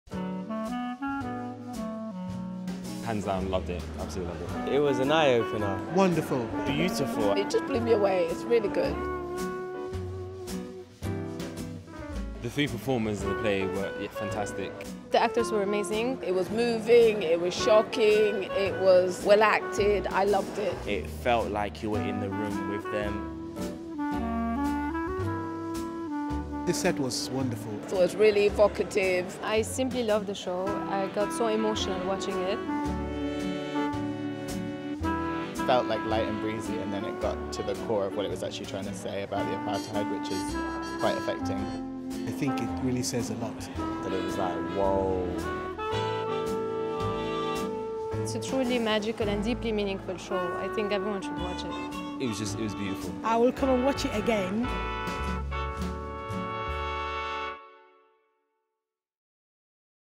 Master Harold and the boys | Official Trailer
The award-winning play Master Harold and the boys (stylised ‘Master Harold’... and the boys) has been officially revived for the National Theatre, where it is now showing in the venue’s Lyttelton auditorium. Written in 1982 by South African playwright Athol Fugard, Master Harold is a politically-charged, thought-provoking piece that dared to oppose the then-controversial apartheid, a cruel segregation system in South Africa from 1948 to the early 1990s that was akin to authoritarianism and deeply rooted in white supremacy. Backed by a fleet of five-star reviews and multiple major theatre awards, Master Harold play tickets are subject to incredibly high demand!